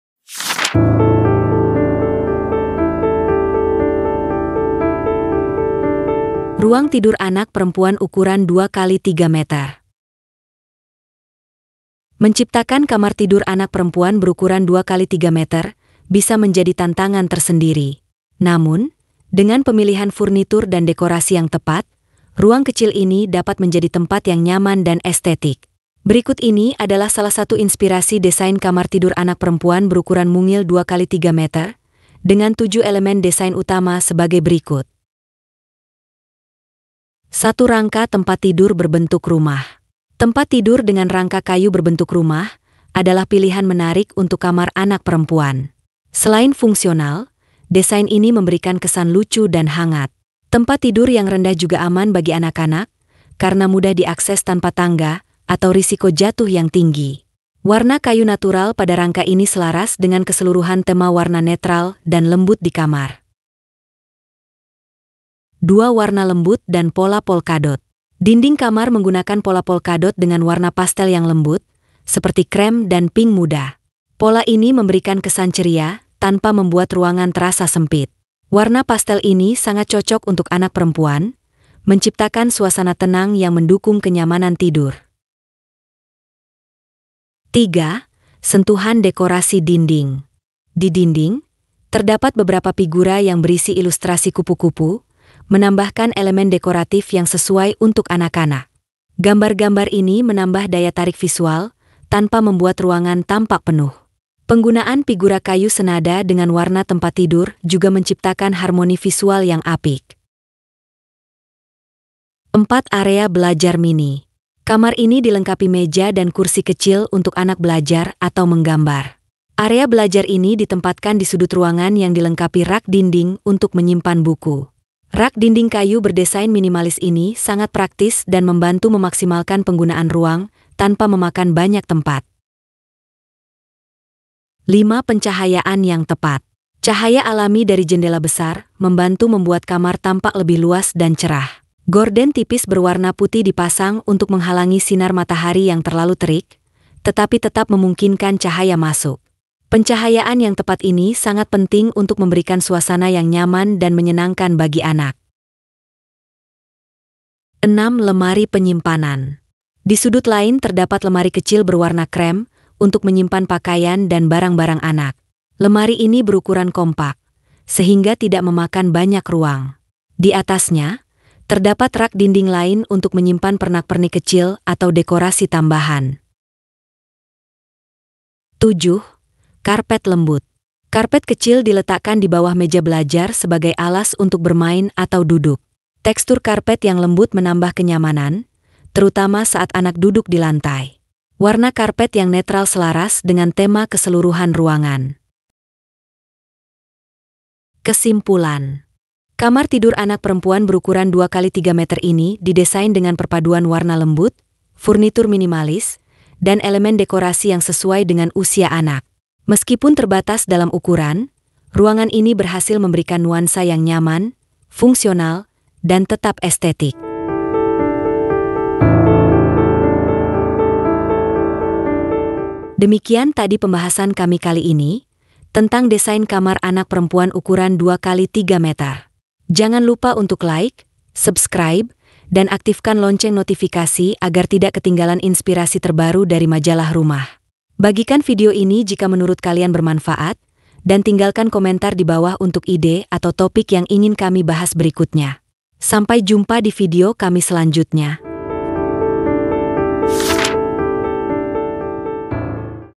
KAMAR ANAK CEWEK 2X3 METER
Membangun kamar tidur anak perempuan berukuran 2 x 3 meter bisa menjadi tantangan tersendiri. Namun, dengan pemilihan furnitur dan dekorasi yang tepat, ruang kecil ini dapat menjadi tempat yang nyaman dan estetik.